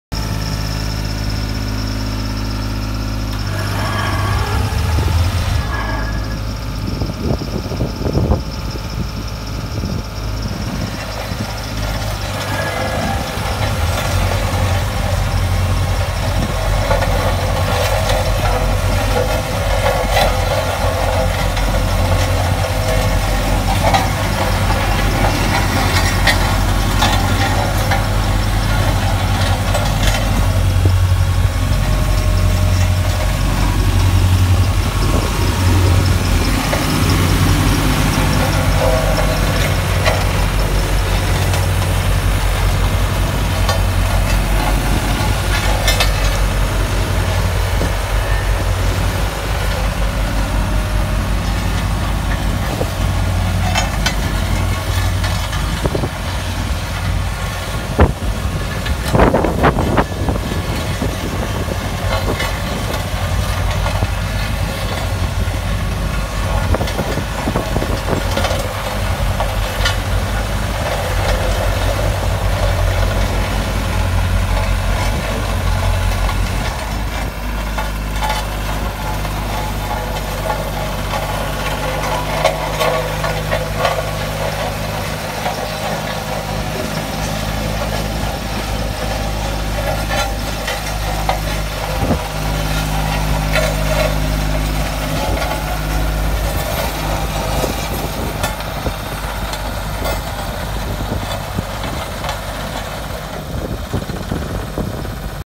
Дорожный каток Bomag BW120 ADH
Тандемный каток Bomag BW125 был завезён в июле месяце с Голландии.

Вес катка 3,5 тонны

Состояние очень хорошее!
Достаточно тяжелый каток, за умеренную цену.

Вся техника доступна в лизинг!

Обращайтесь!